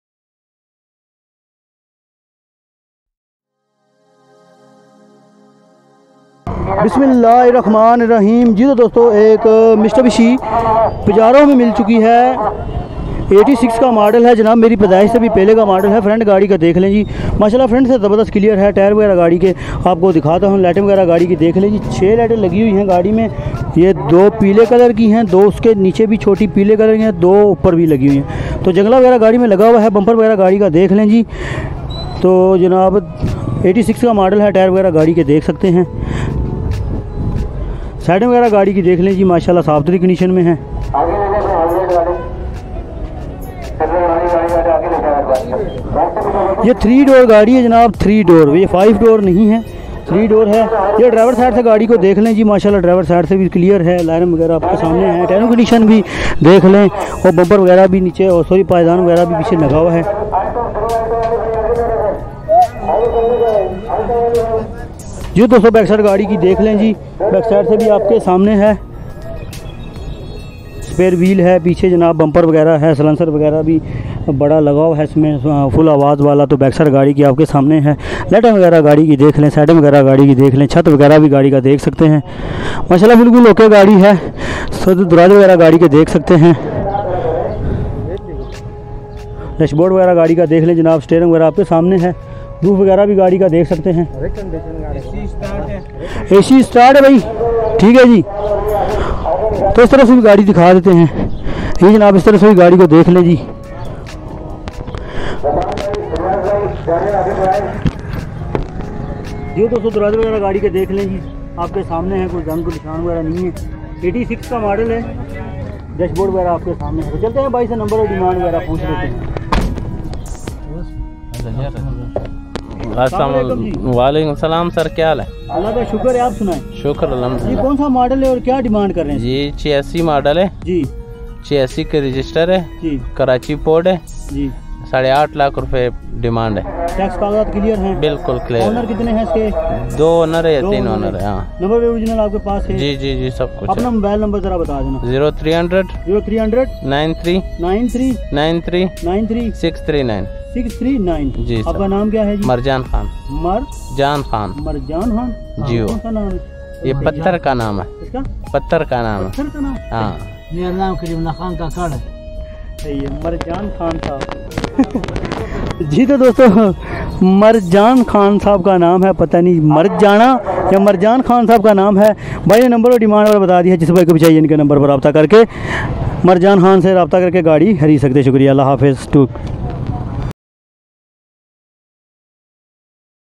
Pajero For sale Model 1983 |Sunday Car Market Karachi Pakistan 08-June-2022 | Ali Bhai 2.2
Sunday car bazaar new karachi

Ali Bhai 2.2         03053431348